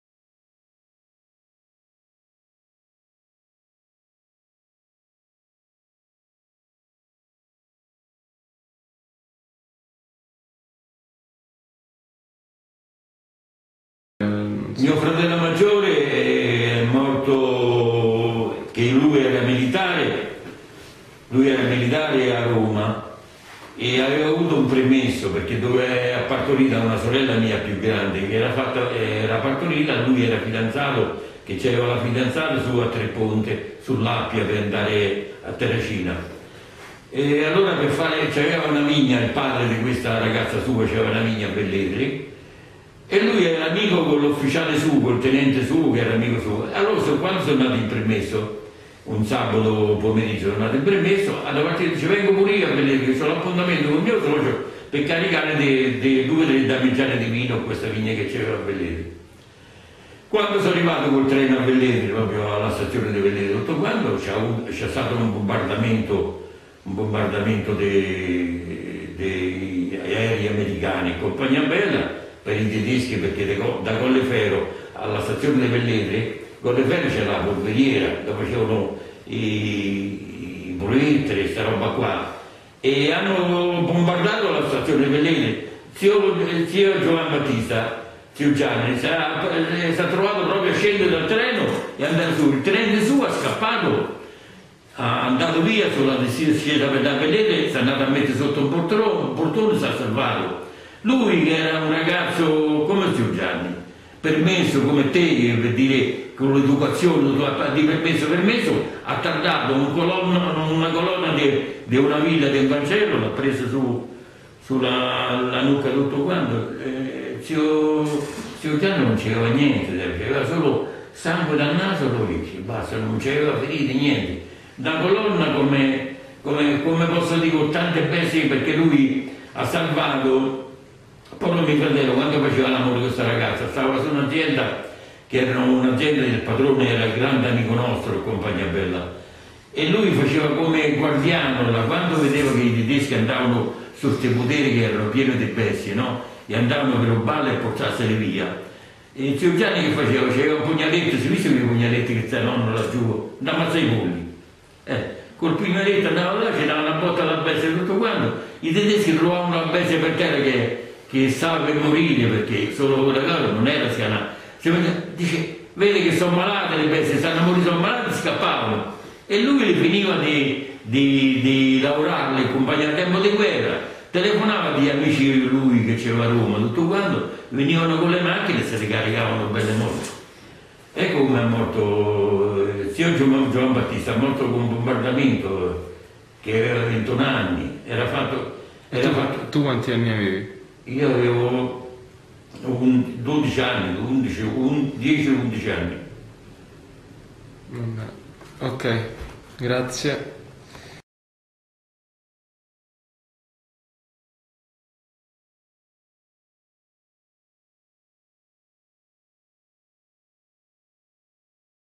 Giulio Gnessi 4 I morti in guerra
Giulio in guerra ha perso il fratello maggiore che era militare a Roma. Era andato da amici a Velletri a caricare una damigiana di olio ma è capitato in mezzo al bombardamento della stazione